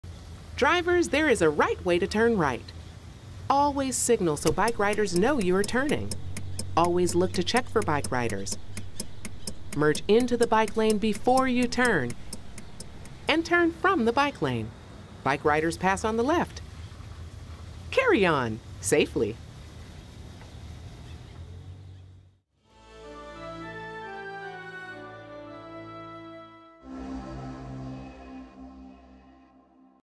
Get Your Right Turns Right
Turning right when a bike lane is present is one of the most dangerous and difficult maneuvers for car drivers on city streets. As part of the VisionZero project the SFMTA provides simple and clear instructions  in this animated example to help drivers perform this difficult procedure.